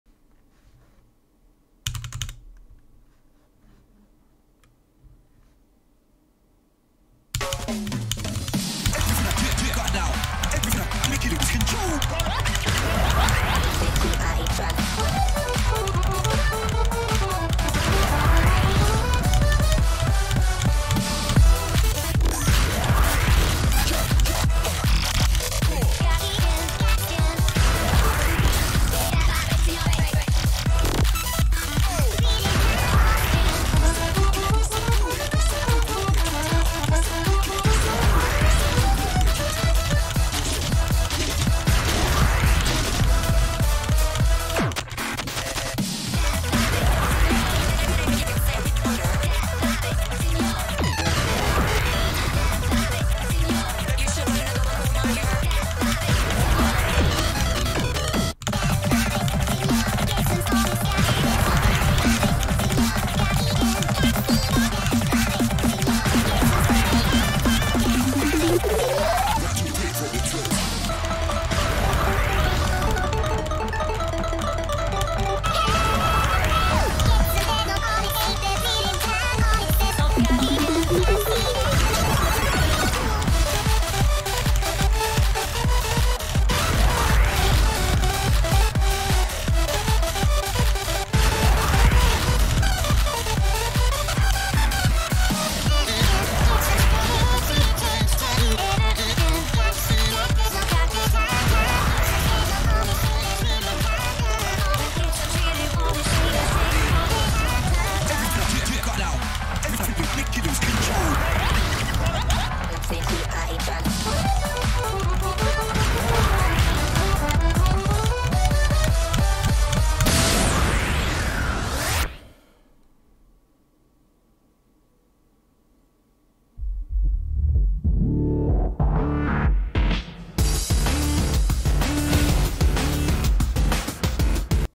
WONDER $LOT 777 but i tried to record the sounds of the keys but it mostly got drowned out
oops